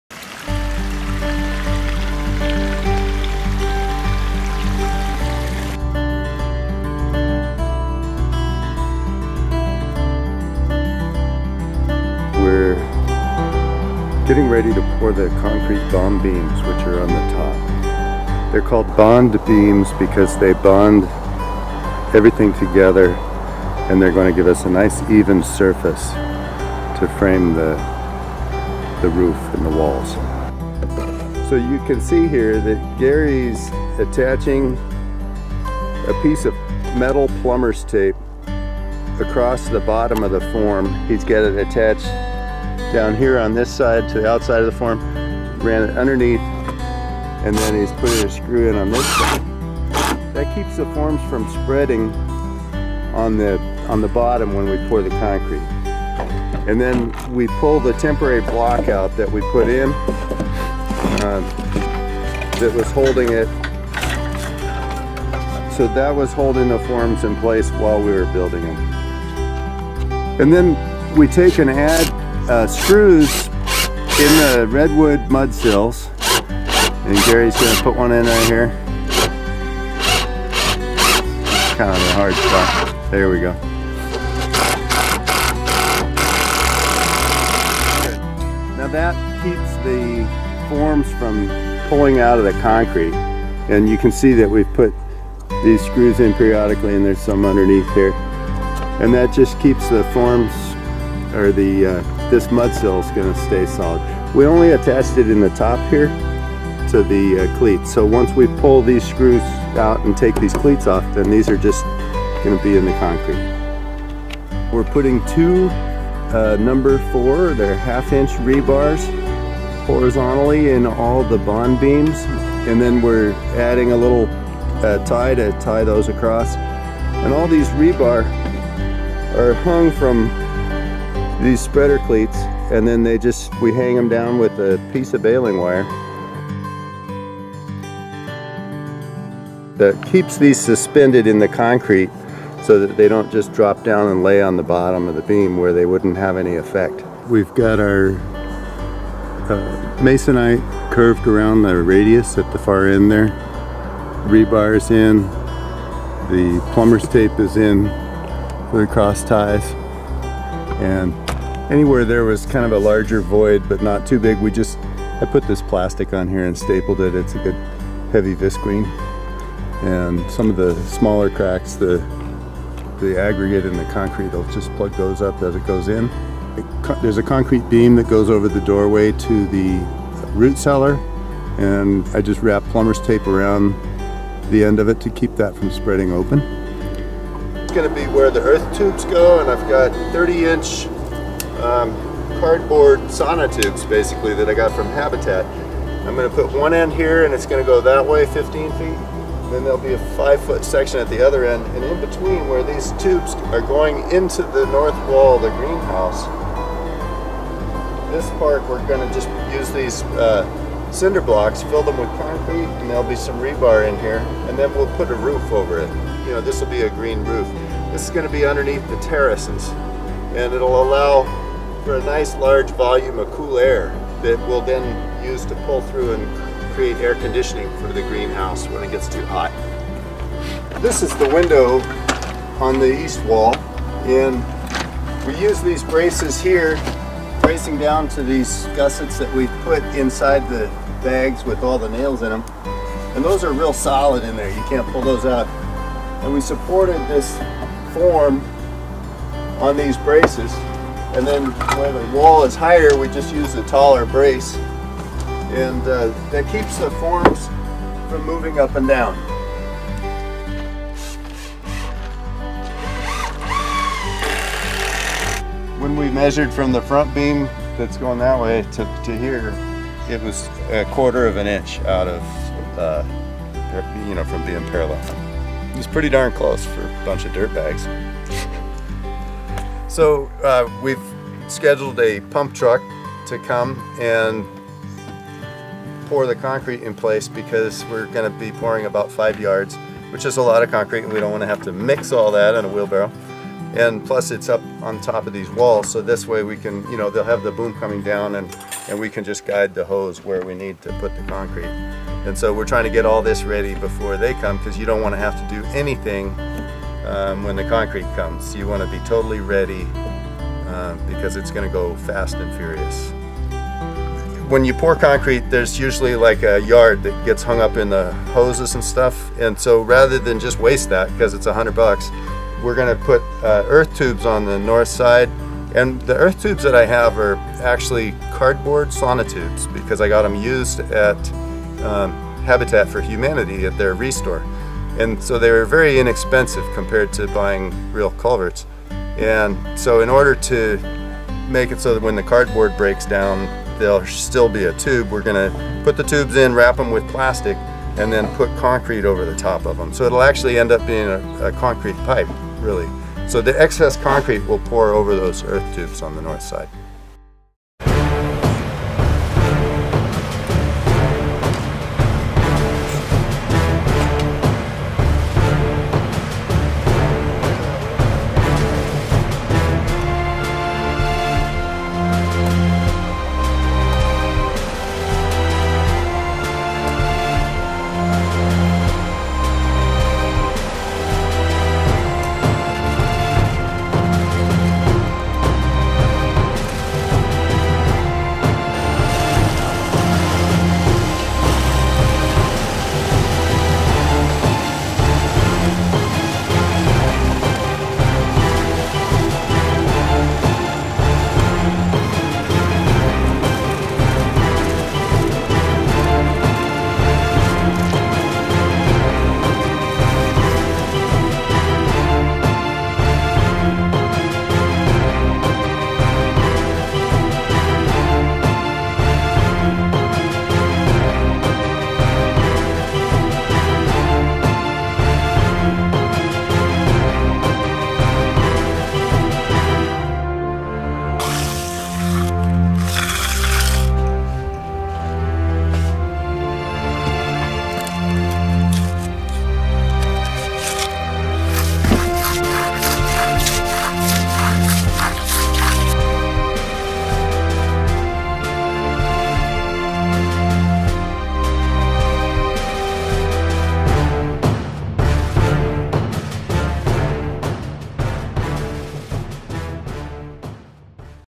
Solar Earthbag, Aquaponic Greenhouse - Bond Beams
Watch us as we pour the bond beams on our solar, aquaponic, earthbag, earthbermed greenhouse that uses rainwater catchment, earth tubes for cooling and a climate battery in the floor as well as a rocketmass heater.